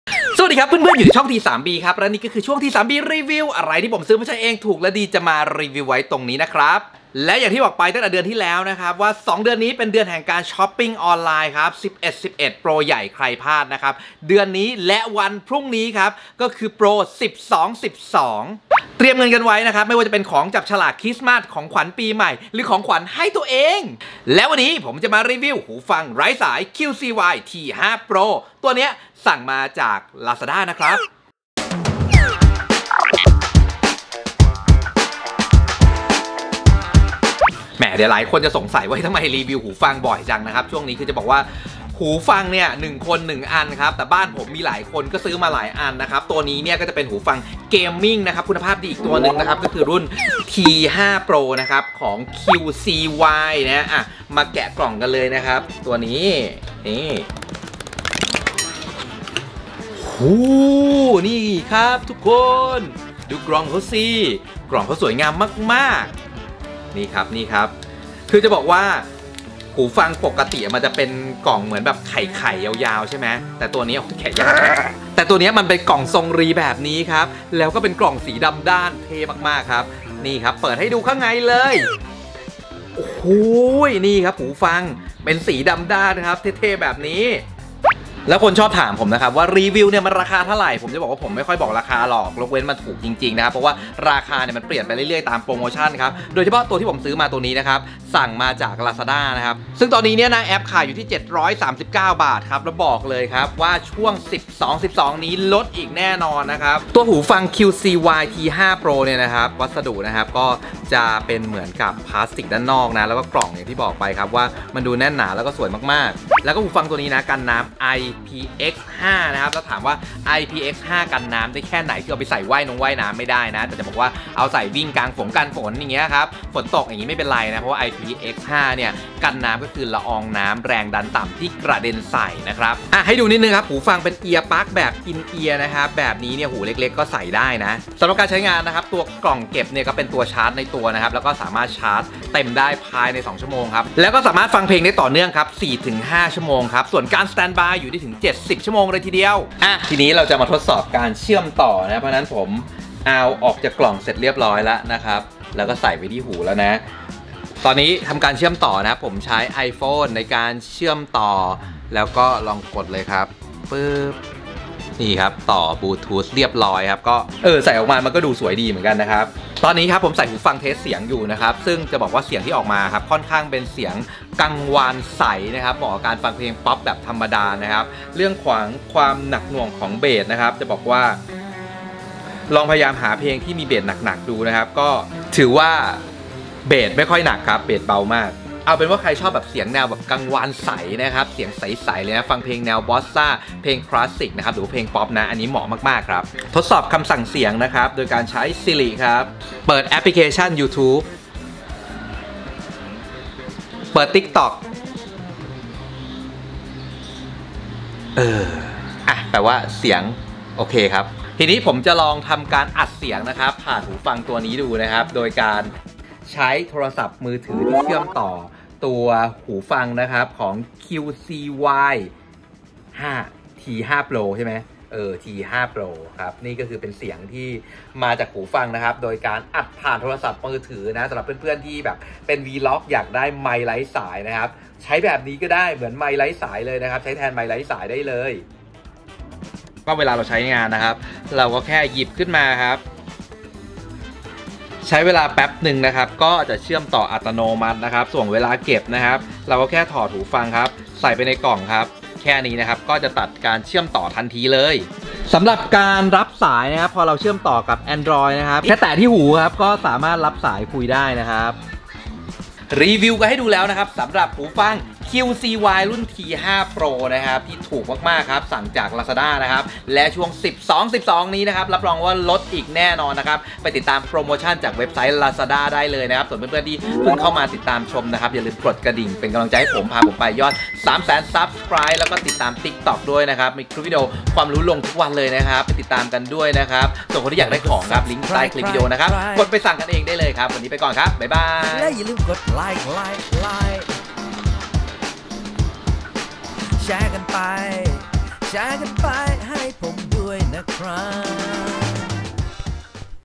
รีวิวหูฟังไร้สาย QCY T5 Pro Gaming l T3B
ช่วง 12.12 Lazada นี้ลดแหลกตามไปซื้อกันได้เลย เสียงดี ไมค์ชัด อัดVDOใช้แทนไมค์ไร้สายก็ได้ ชอบที่สุดคือมันถูกครับ